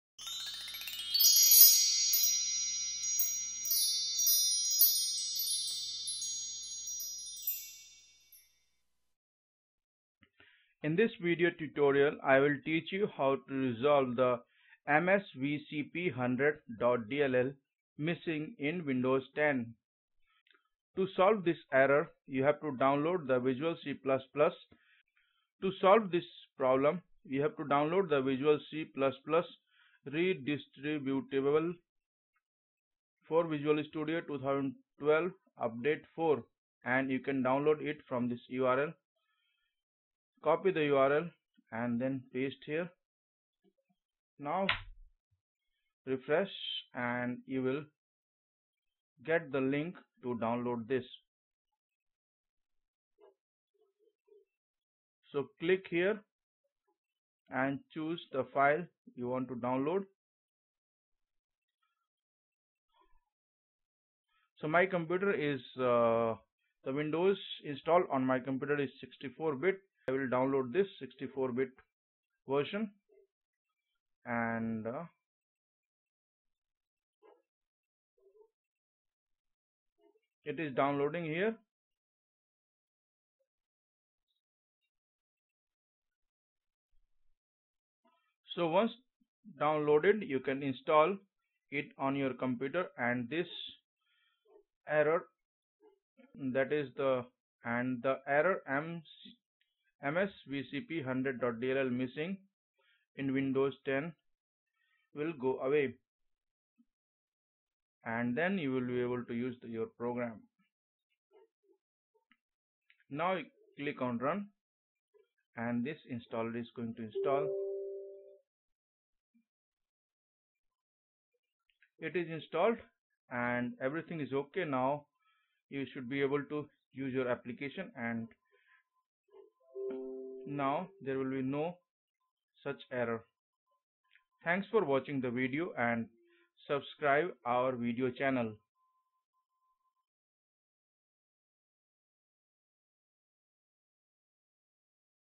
How to resolve the MSVCP100.dll missing in windows 10?
In this video tutorial I will show you How to resolve the MSVCP100.dll missing in windows 10? For solving this download the "Visual C++ Redistributable for Visual Studio 2012 Update 4" from:
And install it on windows 10, this will solve the error.